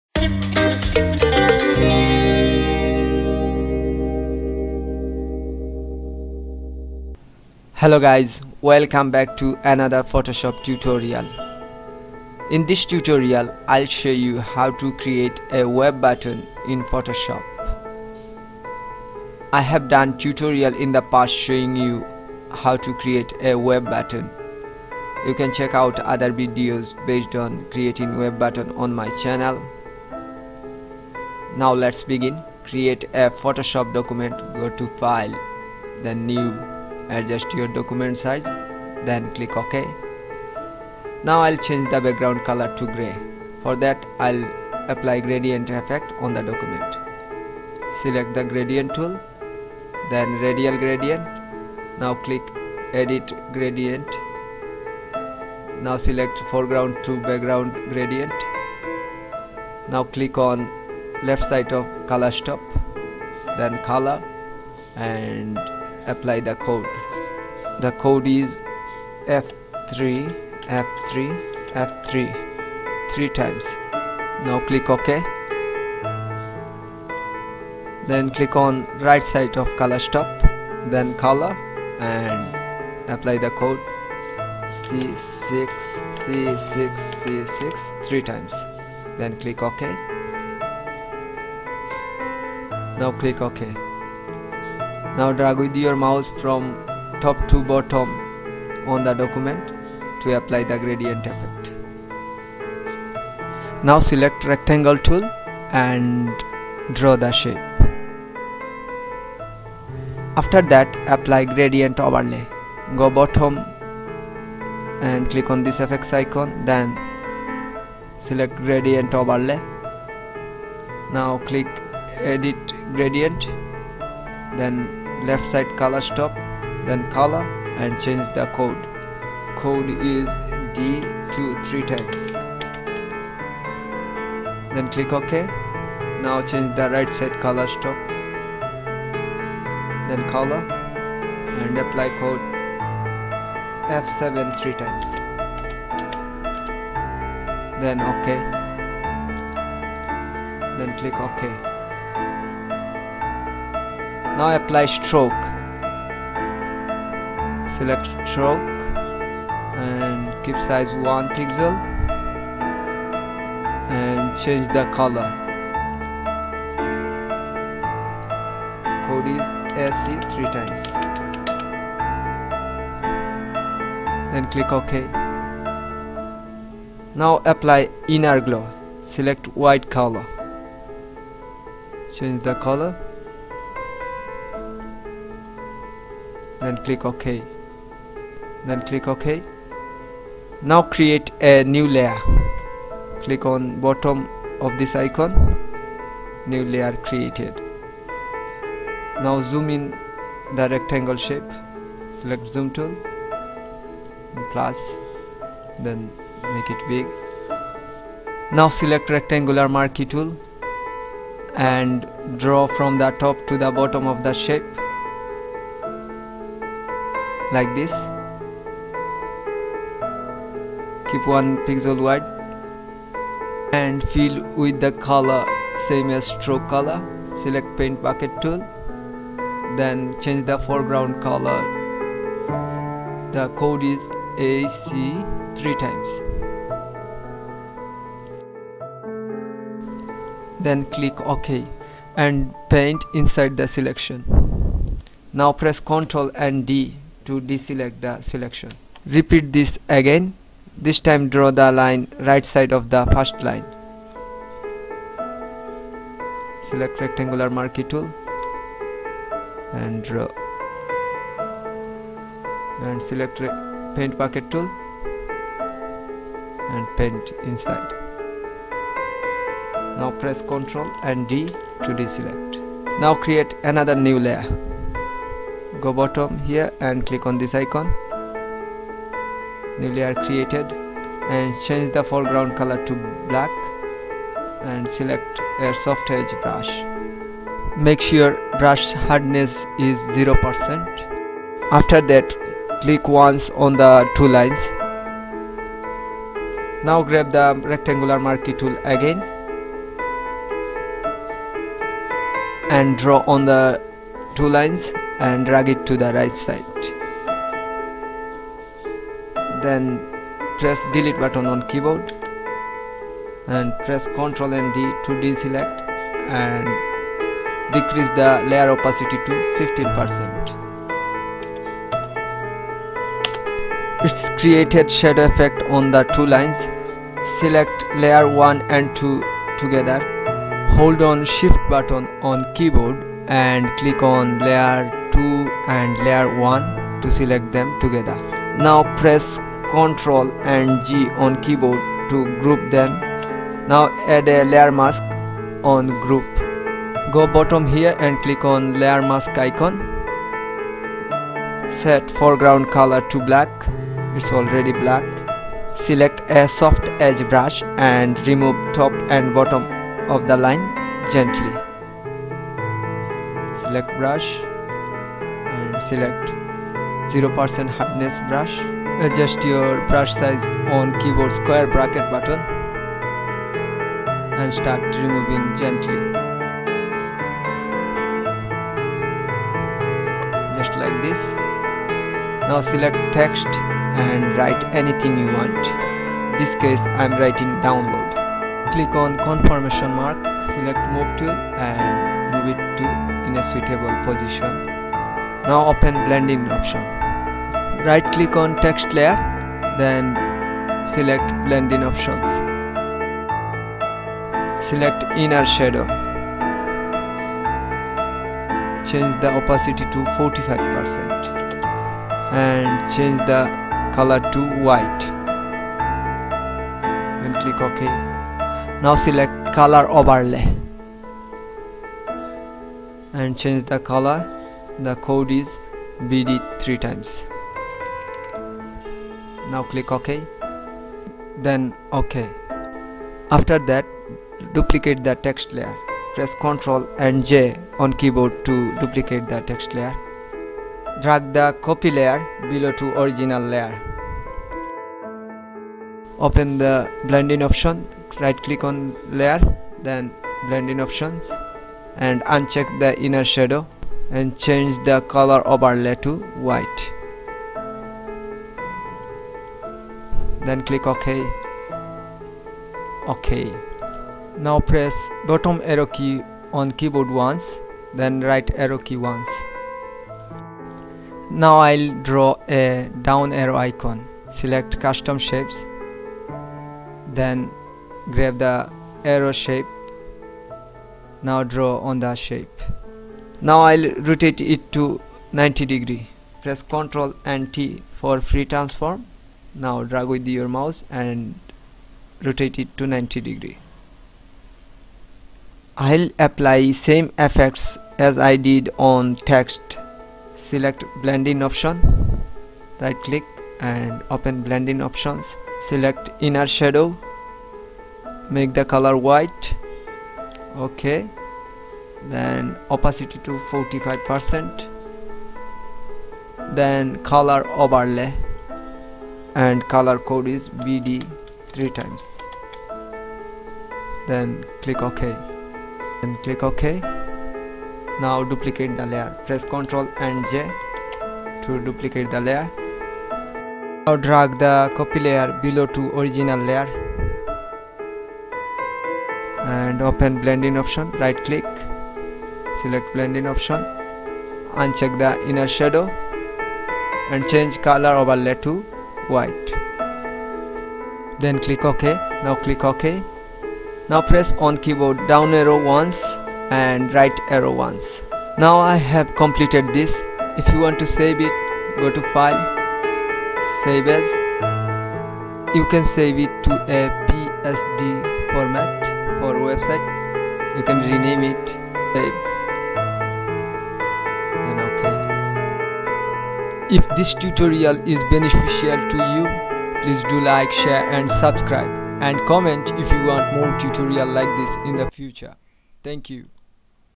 How to create web Buttons in Photoshop
This is another web button tutorial and it is by another style and looks, if you are looking to create your own designed website buttons then you can watch this video and create same like this one or you can modify this and its look, it is very handy way to make web site button,
If you are Photoshop beginner you can try this to boost your Photoshop skills
It’s simple but it has gorgeous look, try this video I have explained everything by this video
And if you apply this method in your other photoshop design it can be beneficial for you